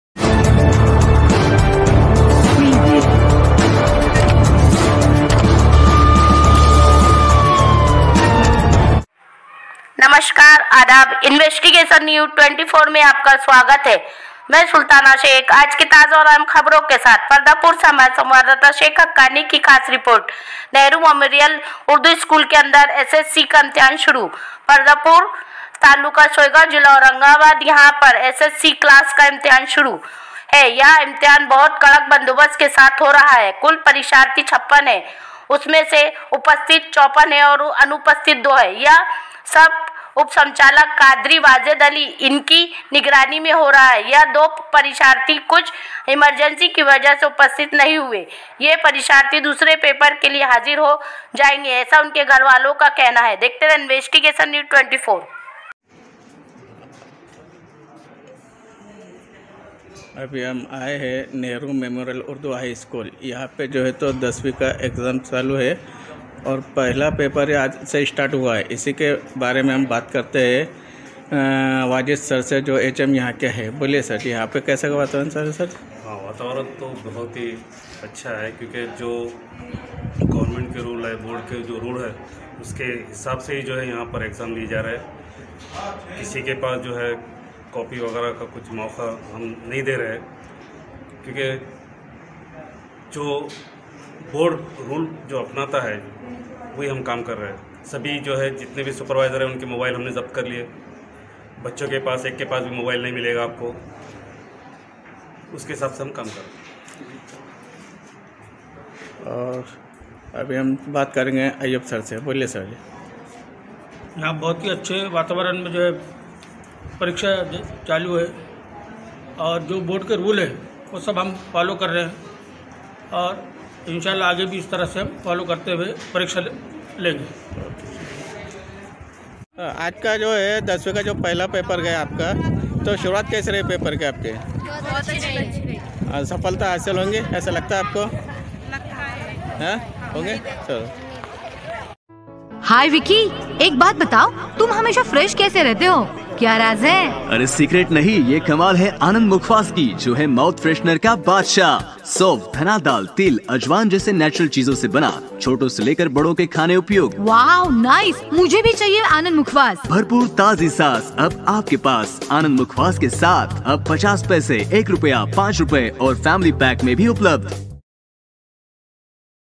फर्दापुर तालुका सोयगांव जिला औरंगाबाद यहां पर दसवीं क्लास का इम्तिहान शुरू है
नेहरू मेमोरियल उर्दू हाई स्कूल के अंदर

 एस एप्स सी का इम्तिहान शुरू

 फर्दापुर तालुका  सोयगांव जिला औरंगाबाद यहां पर दसवीं क्लास का
 इम्तिहान शुरू है  यह इम्तिहान बहुत
 कड़क बंदोबस्त के साथ हो रहा है
 एकूण परीक्षार्थी 56 है उसमें से
 उपस्थित  54  अनु उपस्थित  02 है
 यह सब उपसंचालक कादरी वाजेद अली
 इनके निगरानी मैं हो रहा है
 यह जो दो परीक्षार्थी कुछ इमरजेंसी की वजह से उपस्थित नहीं हुए
 ये परीक्षार्थी दूसरे पेपर के लिए हाजिर हो जाएंगे ऐसा उनके घर वालों का कहना है